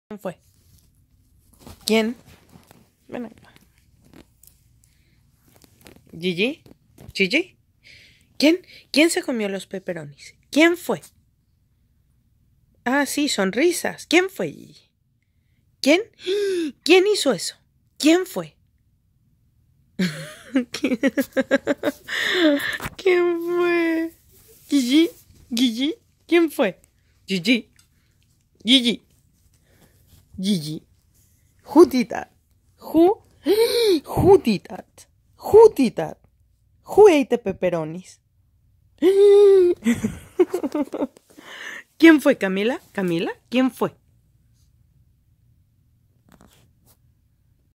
Gigi yorkie smiling because she ate a bag of treats with her sis. Bad girls! Se rie como enojarse
Video from Stephanie K. Ripstein